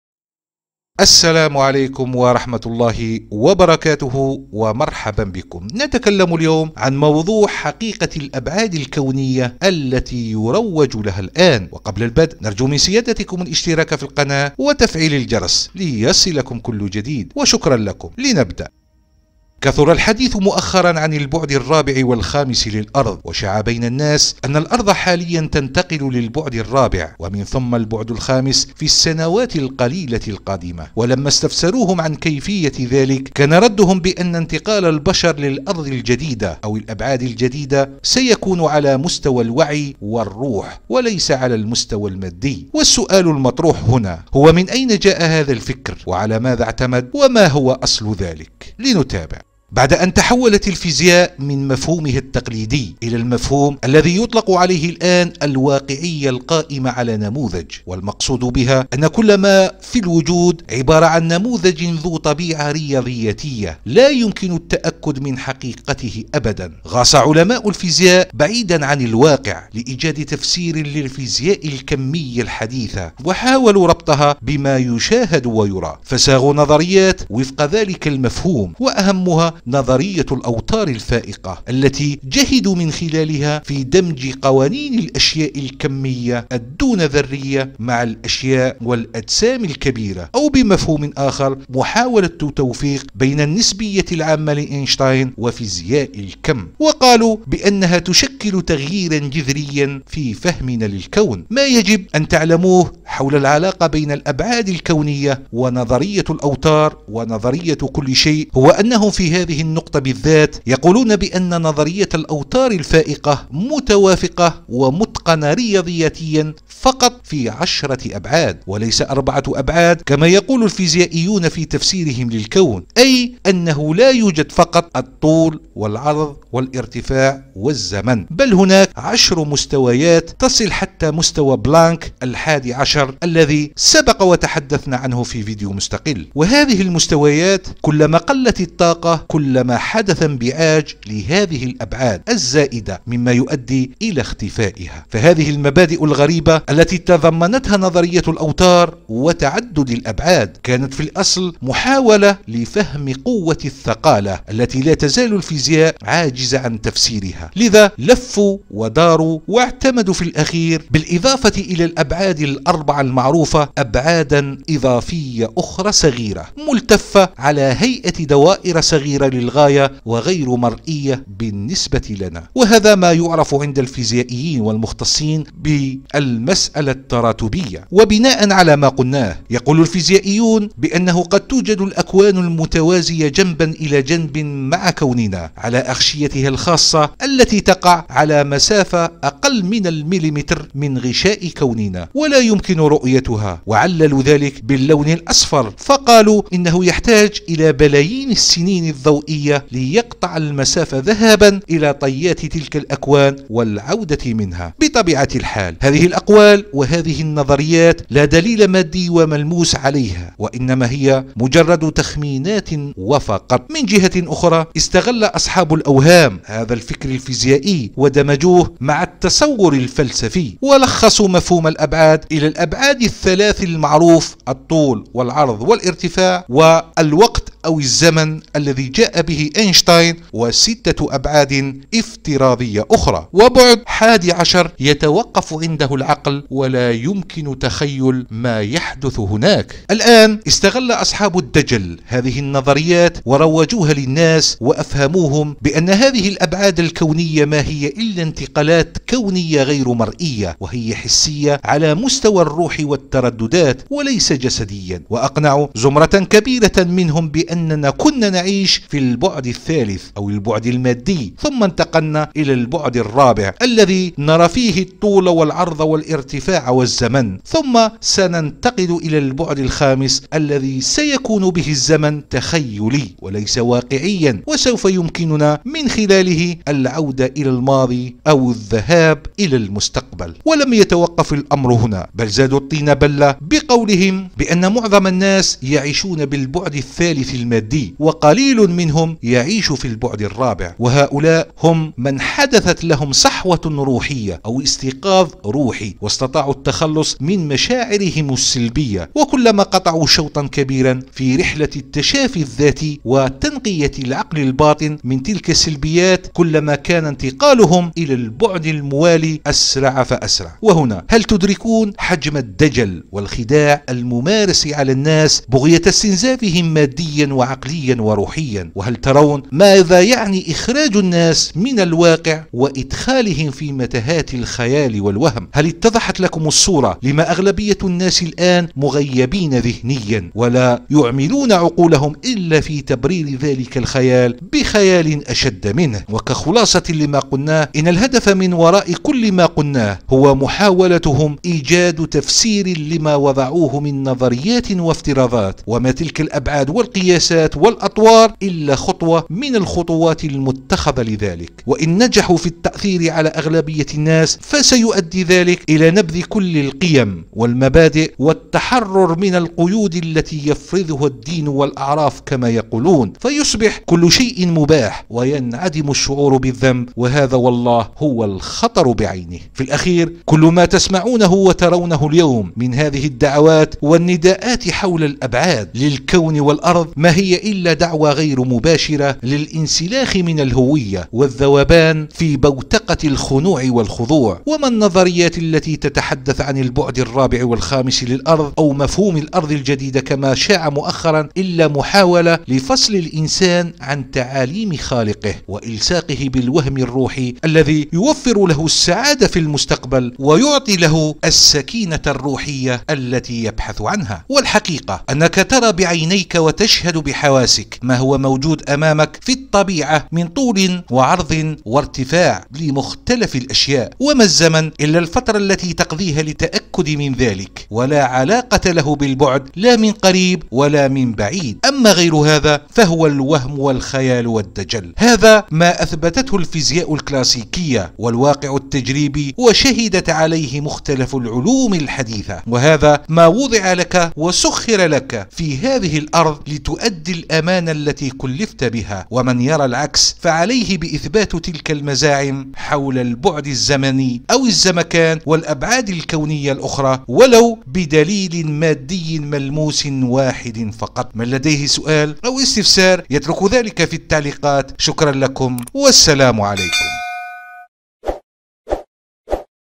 حقيقة الأبعاد الكونية التي يروج لها الآن
حقيقة الأبعاد الكونية التي يروج لها الآن، لقد وجد العلماء أن نظرية الأوتار الفائقة تكون متوافقة ومتقنة رياضيًّا فقط في عشرة أبعاد وليس أربعة أبعاد، كما عهد الفيزيائيون في صياغة تفسيرهم للكون، وحيث إن هذا الوتر يوجد عند مستويات فائقة من الطاقة تعرف بمستوى بلانك، والذي يبلغ مقدار 10 مرفوعة لأس 19 جيجا إلكترون فولت، لذلك فهذه الأبعاد العشرة لو وُجدت فستكون موجودة عند هذه الطاقة الفائقة، وكلما قلَّت الطاقة حدث انبعاج لهذه الأبعاد الزائدة لتختفي من عالمنا الملموس عند الطاقات القليلة نسبيًّا، ليتبقى لنا فقط أربعة أبعاد (الطول والعرض والارتفاع والزمن) الذين يقول بهم الفيزيائيون حتى الآن.
ولقد استغل أصحاب الفكر الروحي هذا الجانب الفيزيائي ونادوا بوجود الأبعاد الحسية والروحية للإنسان وأنه متى كان التخلص من المشاعر السلبية أسرع متى كان الانتقال إلى البعد الموالي أسرع لذا:
تابعوا معنا هذا الفيديو حتى تتعرفوا على هذا الموضوع أكثر وشكرا لكم مسبقا.